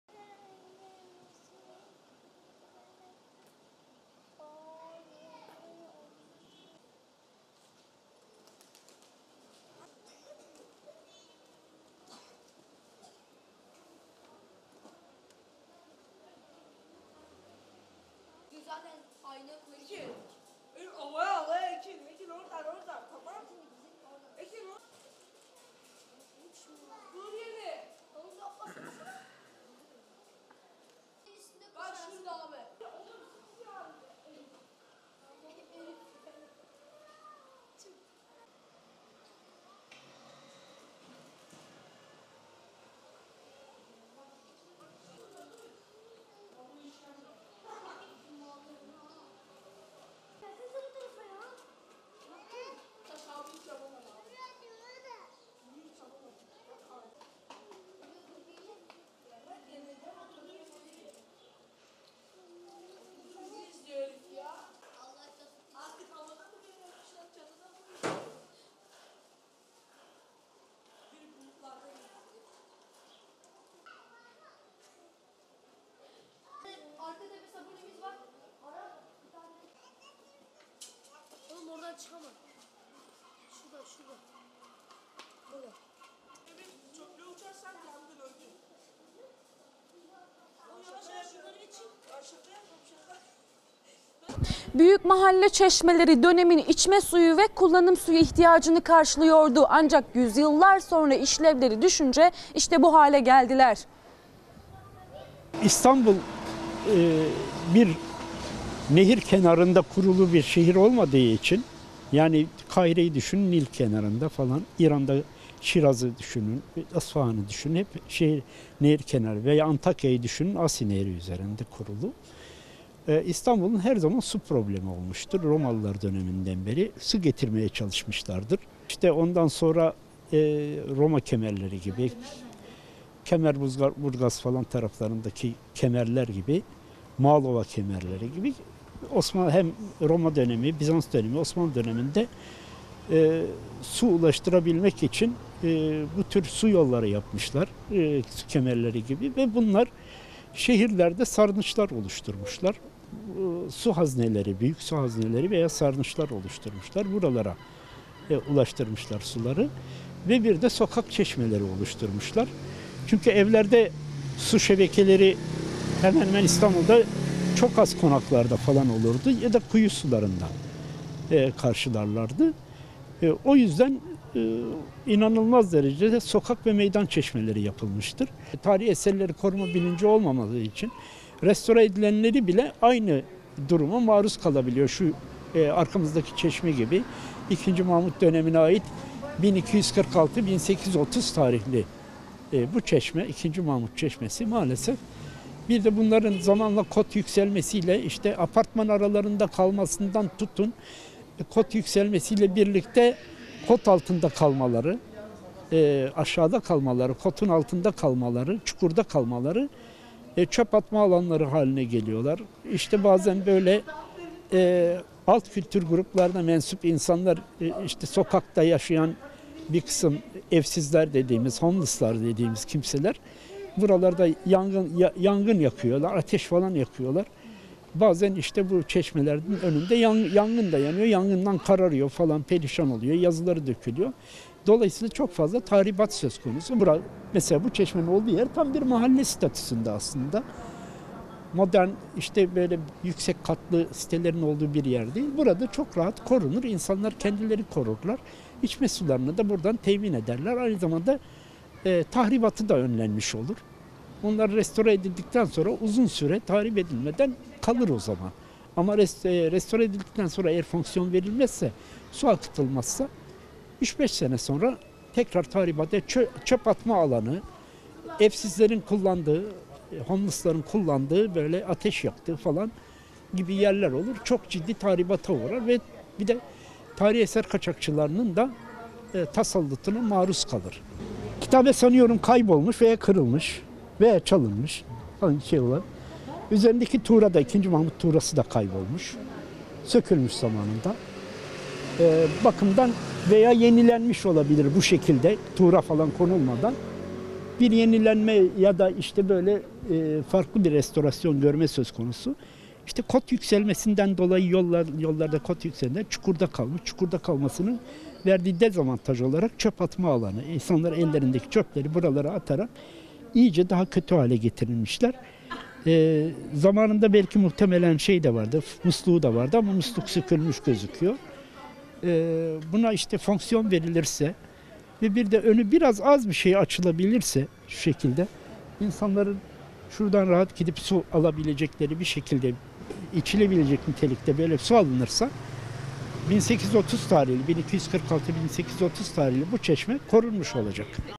Tarihi Çeşme Çöplüğe Döndü
Beyoğlu'nun II. Mahmut tarafından yaptırılan II. Mahmut Çeşmesi'nin çevresi çöplüğü döndü. Sultan II. Mahmut'un tuğrasının da bulunduğu çeşme, çevresindeki yapılaşma nedeniyle yol seviyesinden 3 metre aşağıda bulunuyor. Tarihi çeşmenin korunması için, çevresi demir korkuluk ve tel örgülerle çevrilmiş durumda. Buna rağmen çocuklar çeşmenin üstüne çıkarak oyun oynuyor. Ayrıca çeşmenin hemen önünde de bir çöp konteyneri bulunuyor.
Hasan YILDIRIM - Rojda ALTINTAŞ Özgür EREN / İSTANBUL, (DHA)